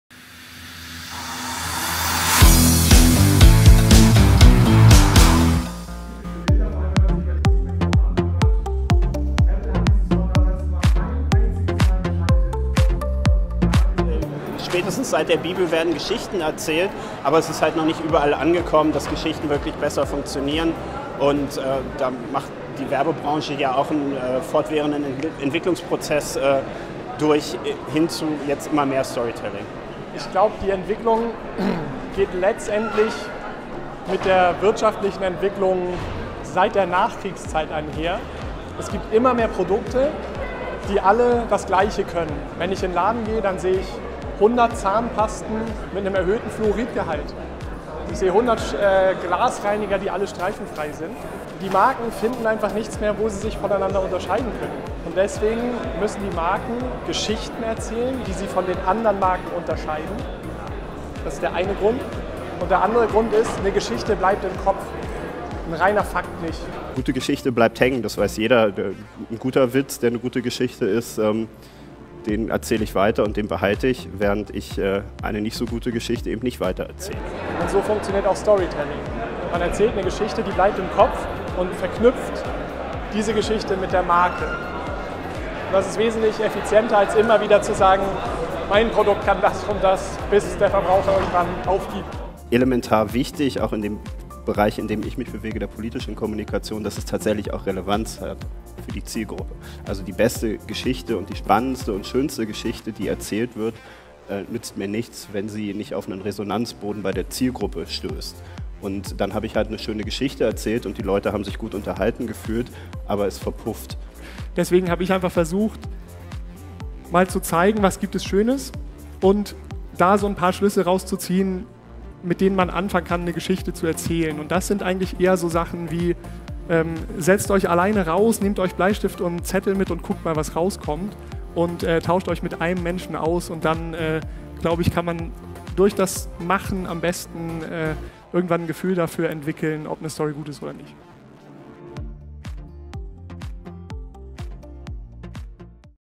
Storytelling in Marketingkampagnen - Medienforum Mittweida 2014
Florian Krumrey (Kreativagentur BUTTER) und Till Felber (Freier Creative Director) erklärten am ersten Kongresstag des Medienforums Mittweida 2014, inwiefern gutes Storytelling in Marketingkampagnen für eine hohe Reichweite sorgen und enorme Aufmerksamkeit bewirken kann.

Kamera: Franz Müller
Ton: Max Glück
Redaktion & Schnitt: Robin Jugel